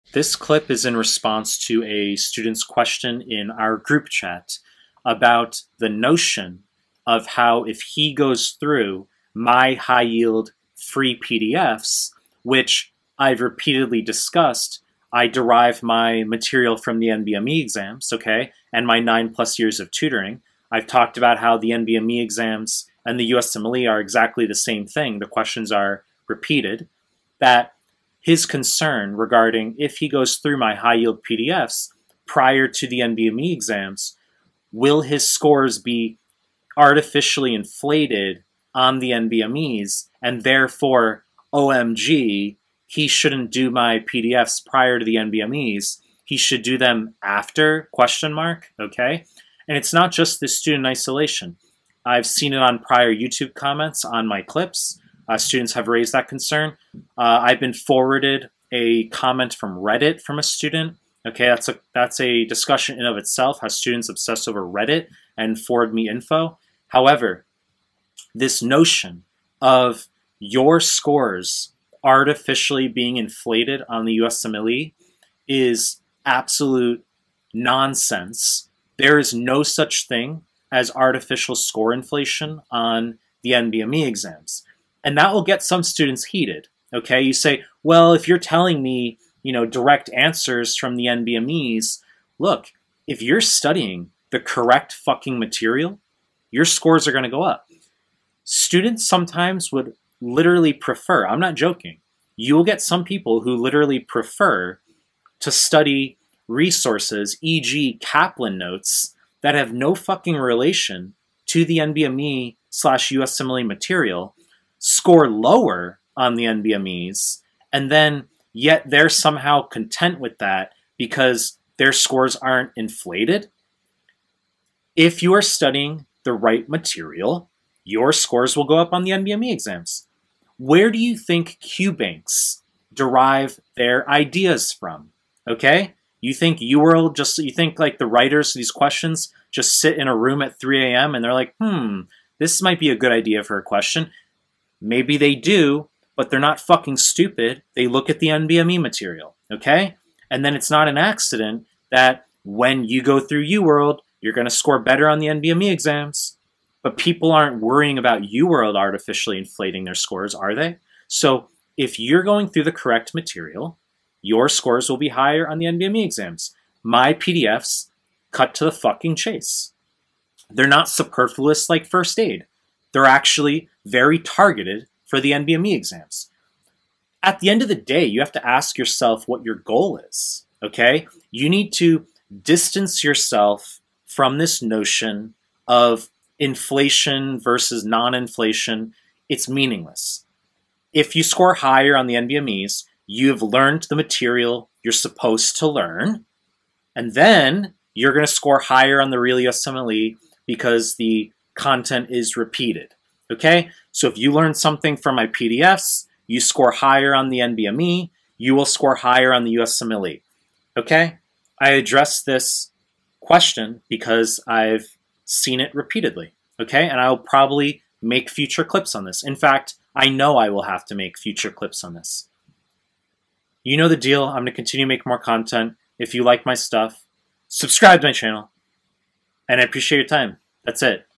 USMLE - "Will Mehlmanmedical HY PDFs artificially inflate my NBME scores?"
In this clip I talk about whether going through the Mehlmanmedical HY pdfs will artificially inflate one's score on the NBMEs. I will be posting various random clips like this to informally address questions you guys have from the Telegram group.